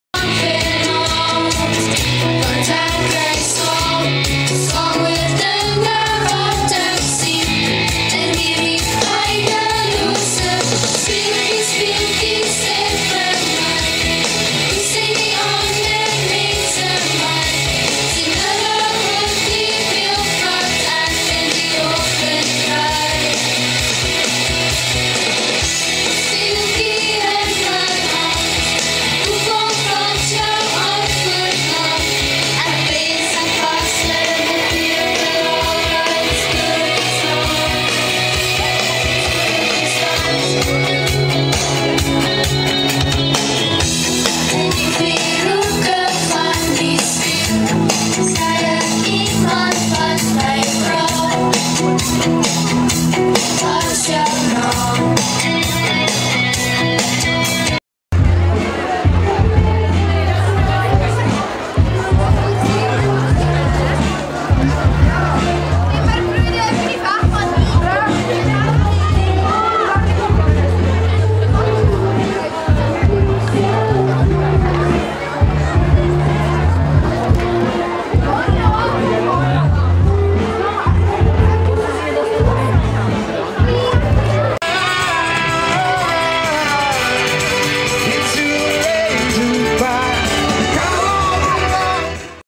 Laerskool Middelburg celebrates 2014 Game Festival
With lots of fun and excitement children and parents had a great time.